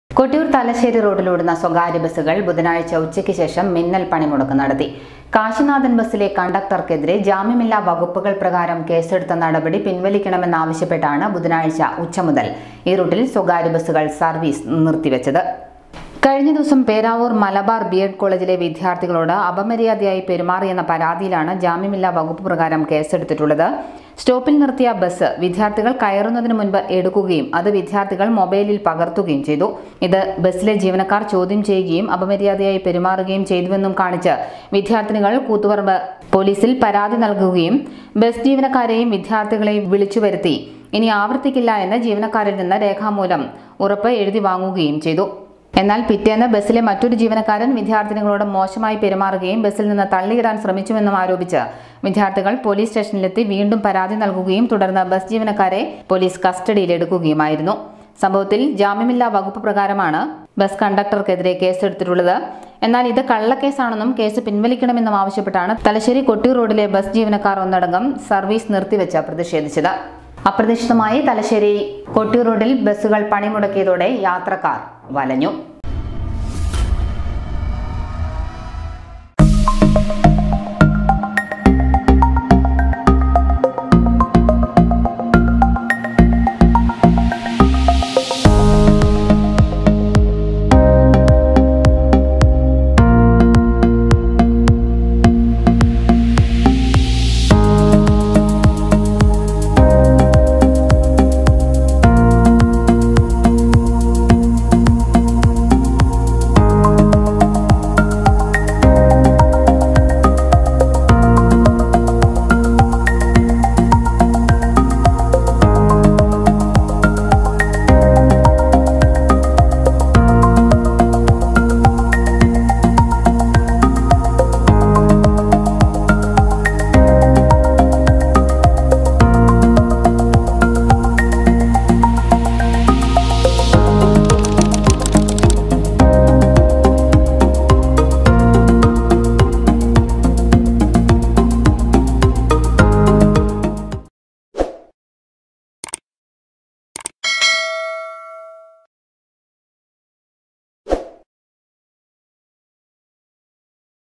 private bus strike kottiyoor thalassery route
കണ്ടക്ടർക്കെതിരെ ജാമ്യമില്ലാ വകുപ്പ് പ്രകാരം കേസെടുത്ത സംഭവം; കൊട്ടിയൂർ തലശേരി റൂട്ടിൽ സ്വകാര്യബസുകളുടെ മിന്നൽ പണിമുടക്ക് - പണിമുടക്കിൽ വലഞ്ഞ് യാത്രക്കാർ